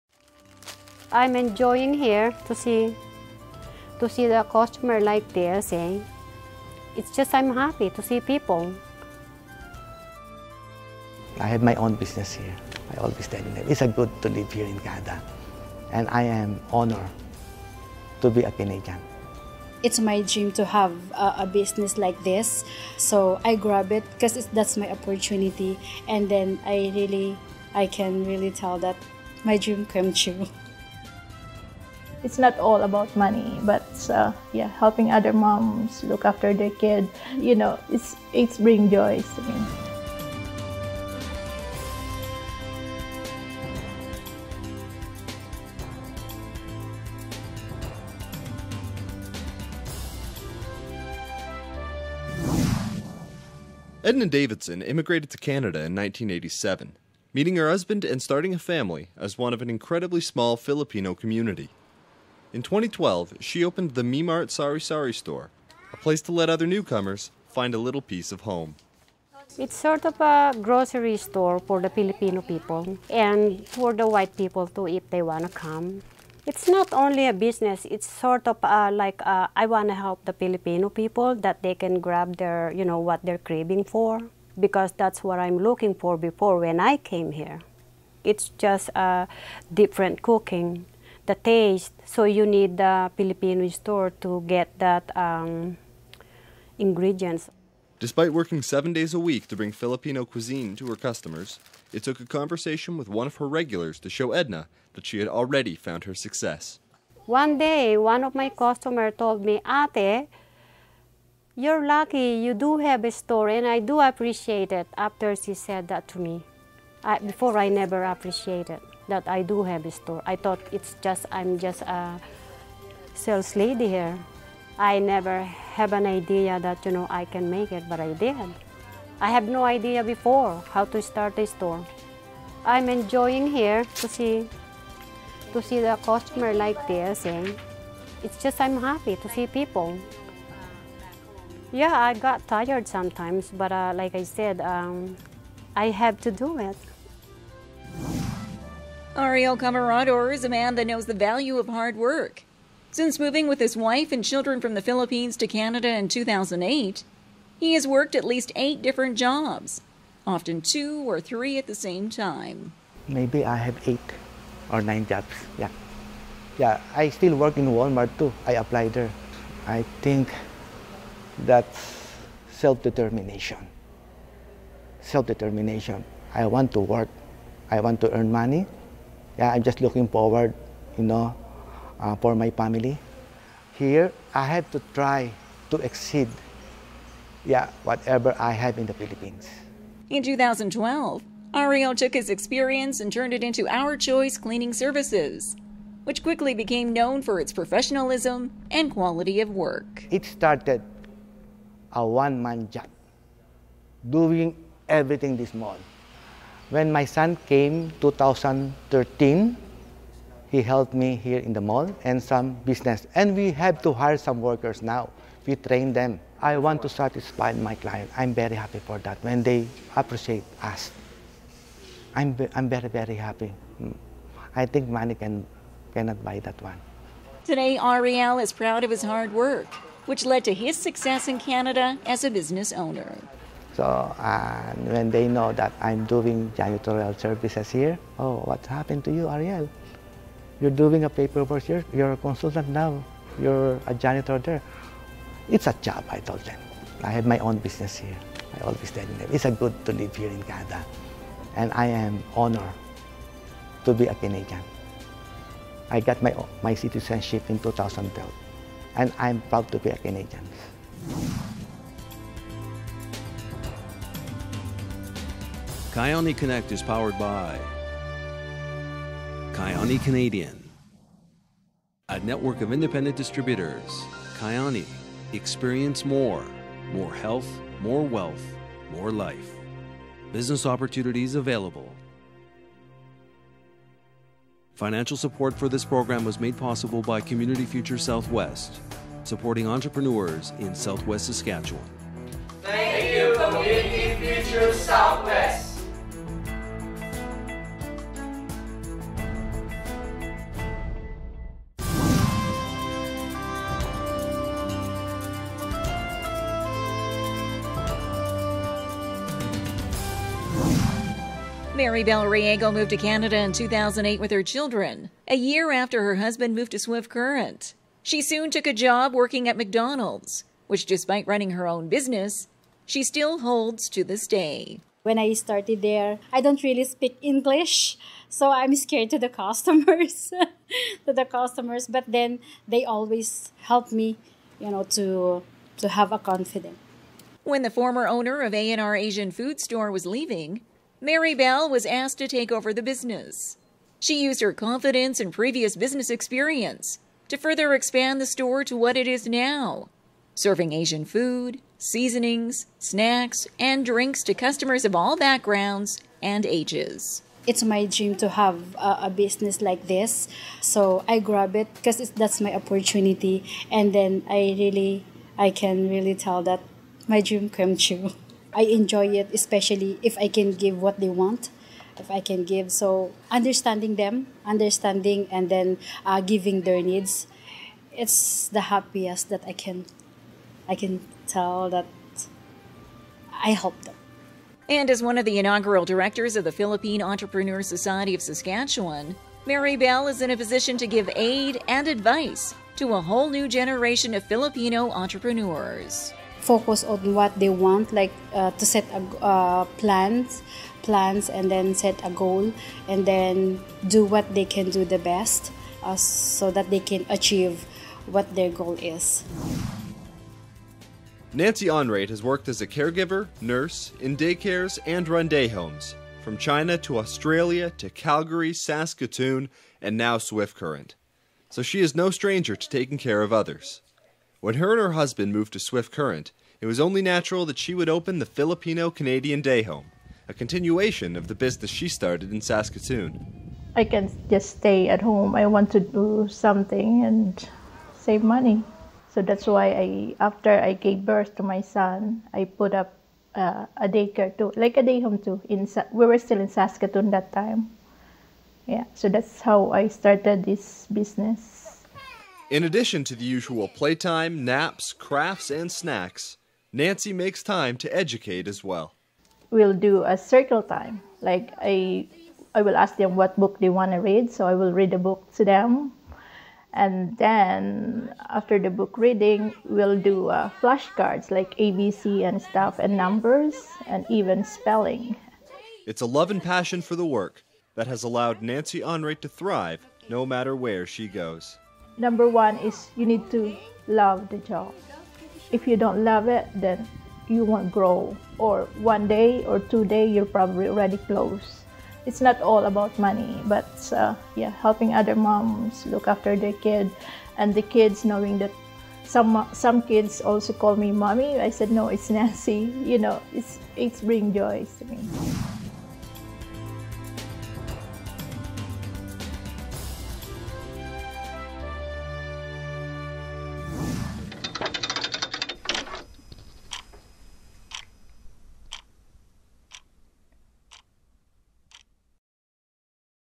Newcomer Entrepreneurs making it in Saskatchewan
This video takes a look at 4 entrepreneurs and their story of being a successful newcomer in Saskatchewan.Meet Nancy Onraet a lady who opens up her heart and her home to local families with day home services.Meet Maribel Riego owner of A & R Asian Food Store she’s an entrepreneur who’s mind is always working looking for ways to add value to her business.Meet Ariel Camarador a man obsessed with providing impeccable customer service. While most people are fast asleep in their warm comfortable beds Ariel and his team at Our Choice Cleaning Services are just getting to work.Meet Edna Davidson a local entrepreneur who has called Swift Current home since 1986. Edna operates Me Mart food store in the city and takes great pride in treating her customers like family.